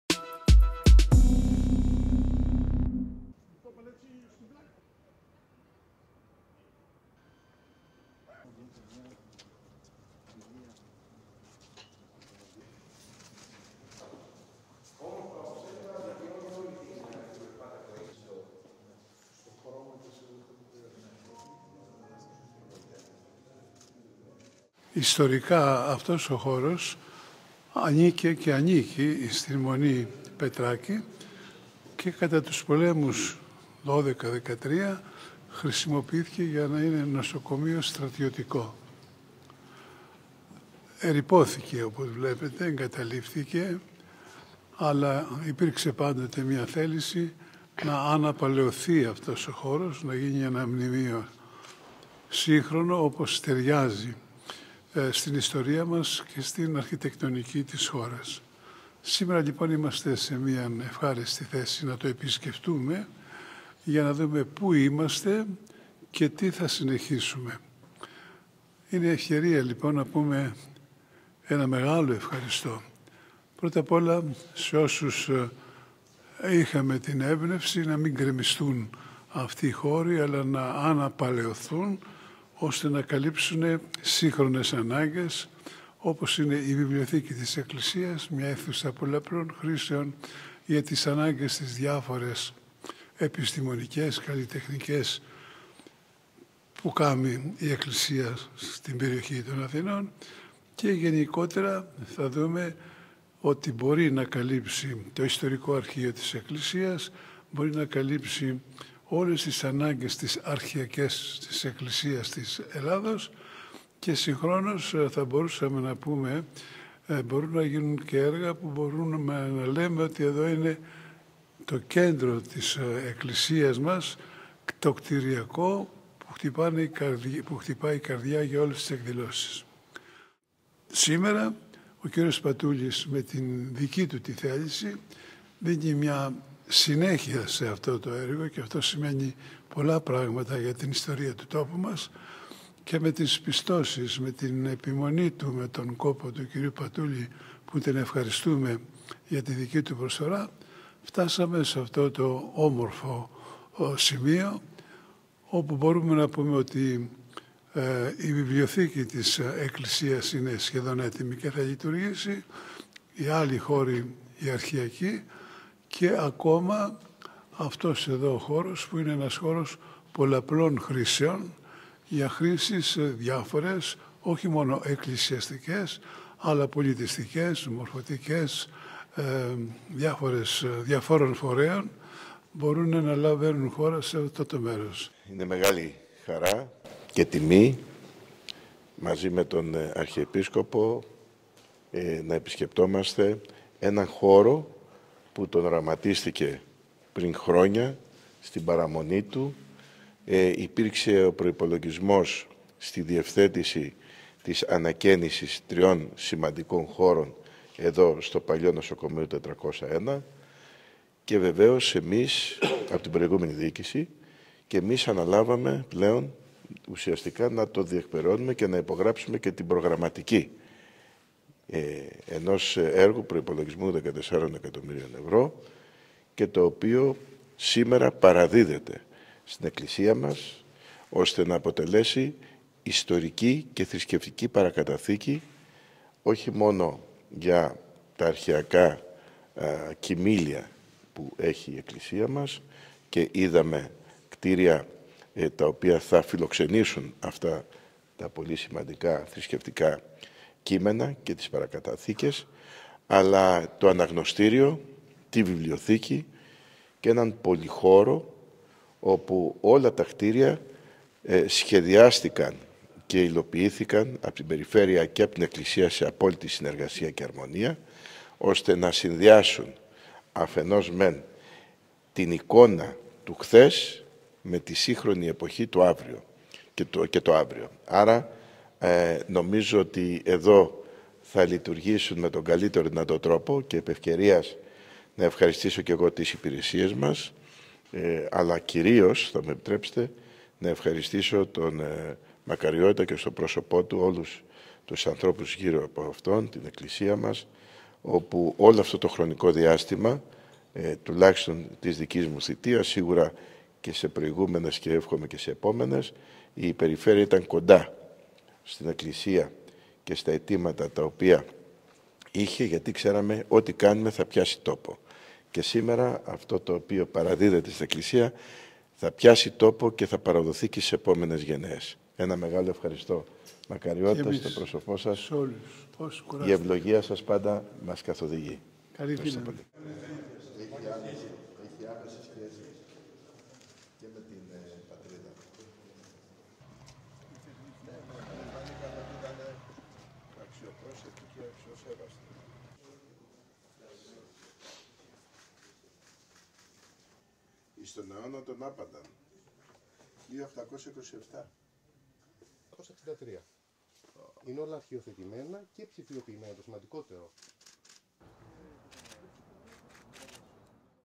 Εγκαίνια των κτιρίων του Οικοδομικού Συγκροτήματος Πρώην 401 ΣΤΡΑΤΙΩΤΙΚΟΥ ΝΟΣΟΚΟΜΕΙΟΥ
Εγκαίνια των κτιρίων του Οικοδομικού Συγκροτήματος Πρώην 401 ΣΤΡΑΤΙΩΤΙΚΟΥ ΝΟΣΟΚΟΜΕΙΟΥ.Πηγή: ΑΠΕ-ΜΠΕ